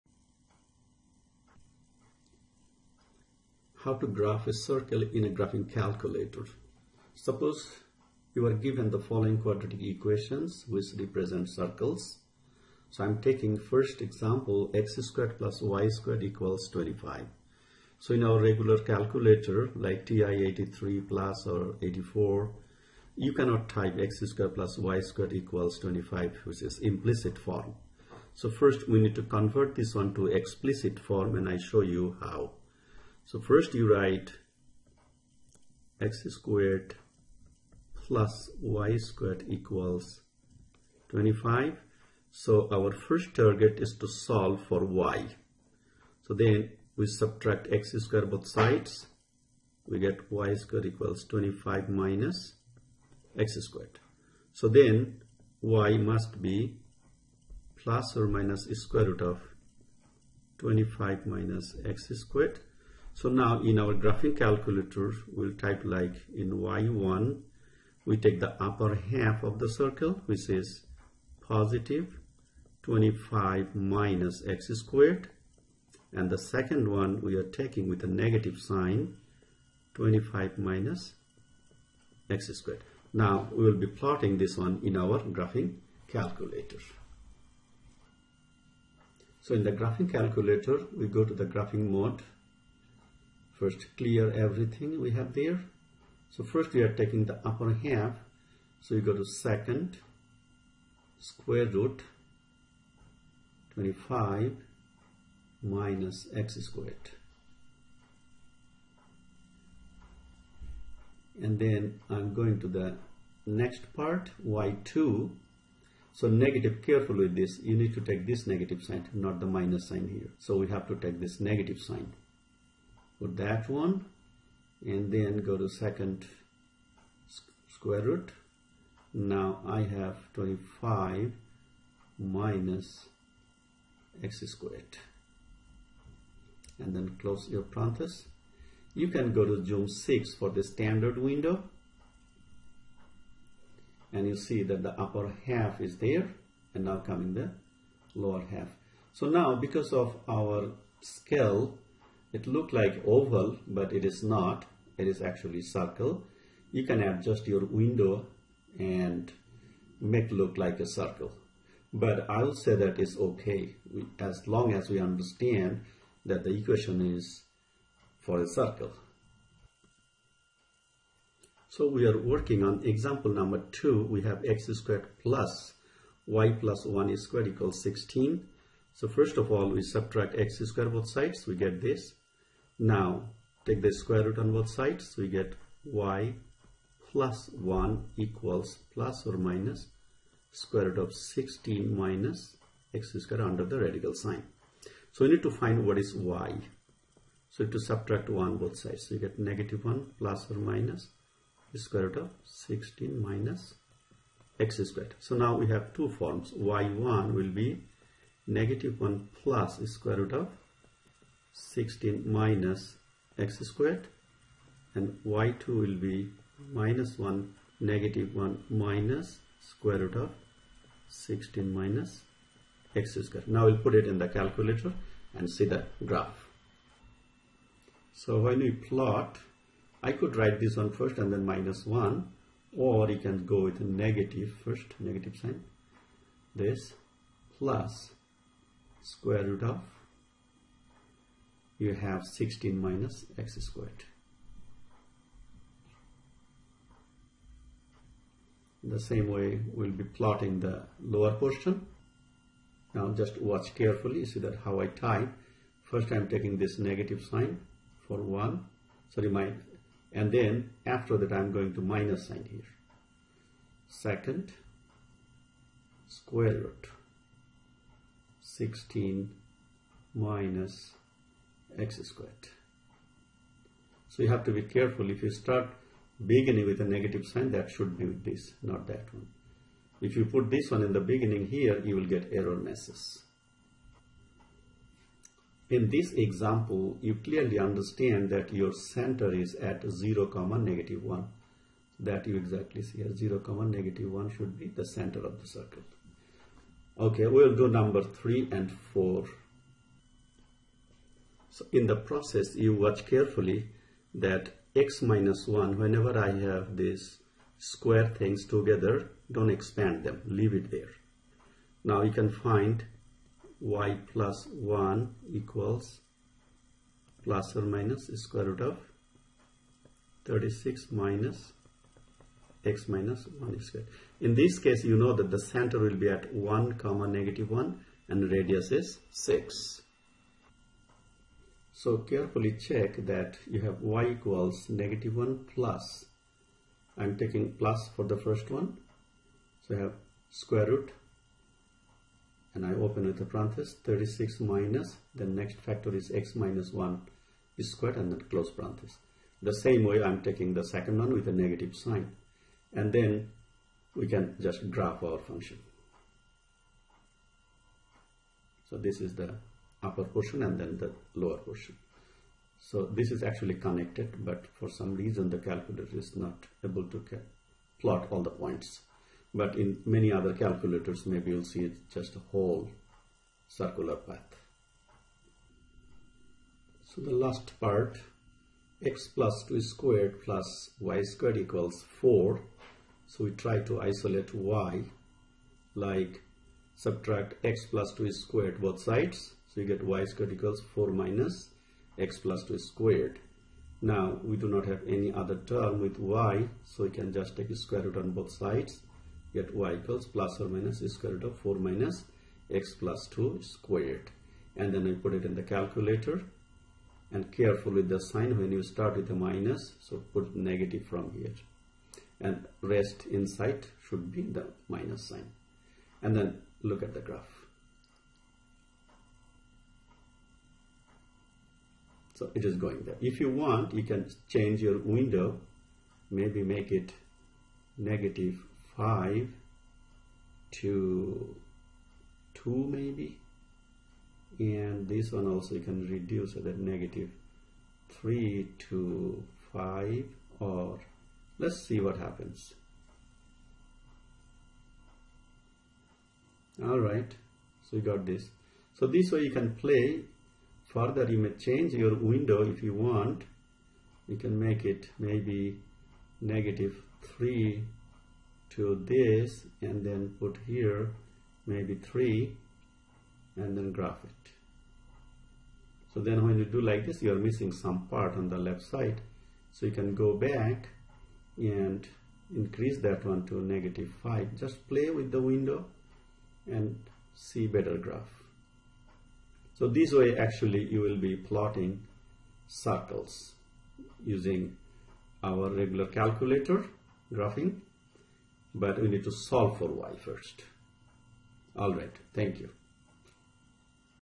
Circle11321
In this video, you will learn how to plot the graph of a circle from its quadratic representation.